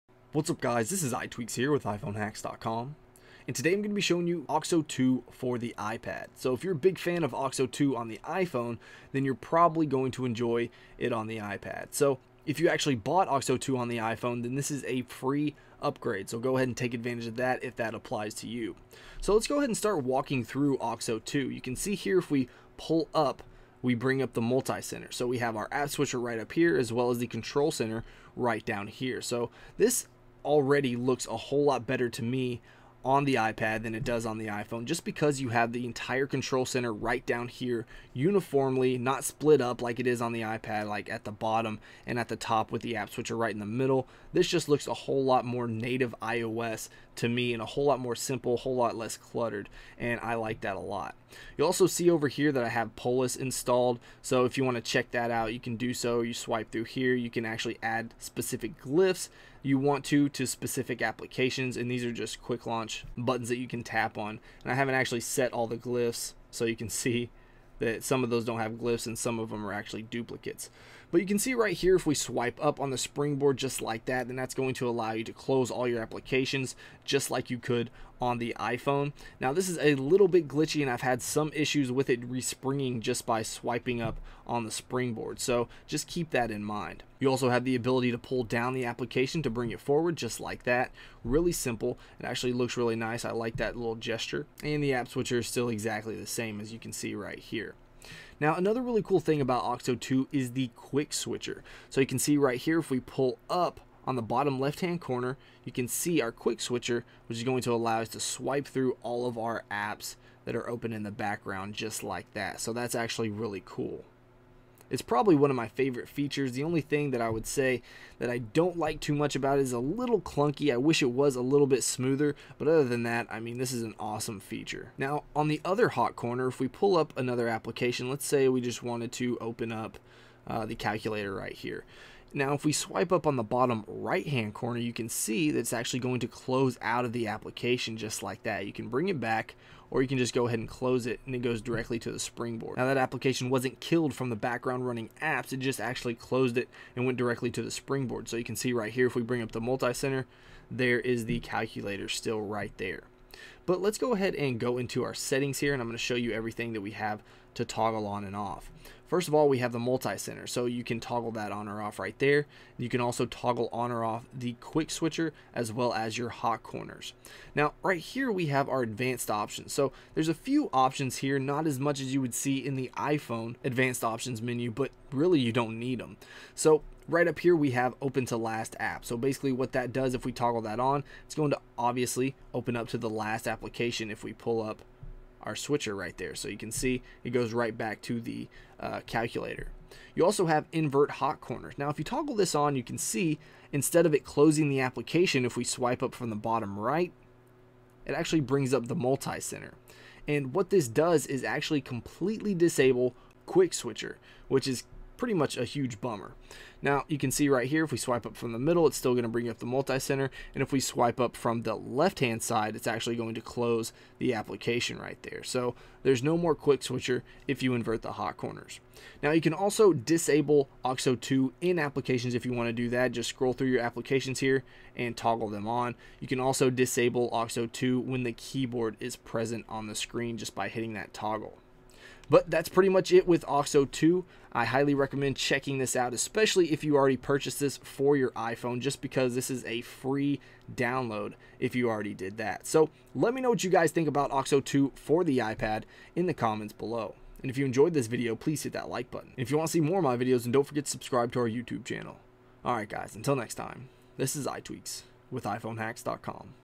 Auxo 2 for iPad Review - iPhone Hacks
Video walkthrough of Auxo 2, one of the best jailbreak tweaks for iPad.